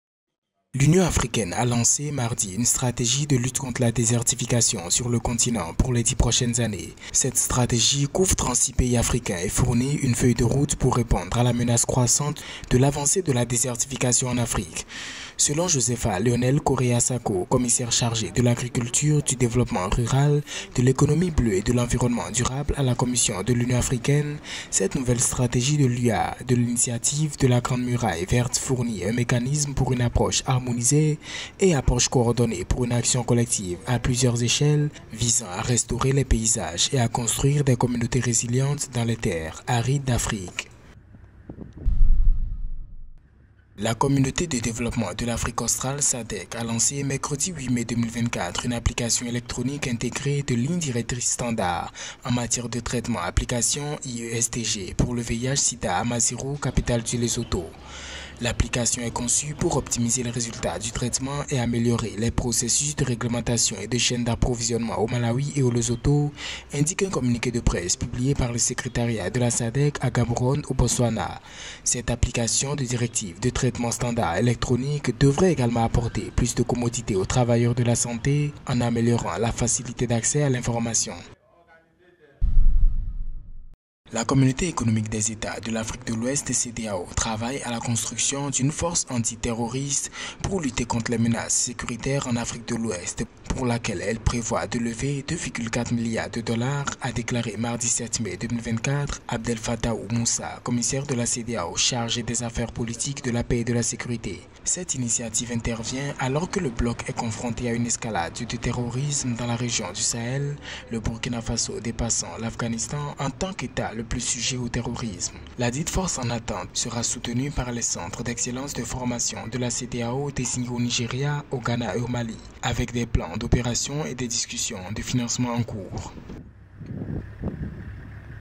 African Union Journal : l’Actu des Régions
Au sommaire de cette édition :
-L'Union africaine lance une stratégie de lutte contre la désertification
-La SADC lance une application électronique intégrée de directives de traitement standard pour le traitement du VIH et du SIDA
-La CEDEAO prévoit un fonds de 2,4 milliards USD pour la création d'une force antiterroriste en attente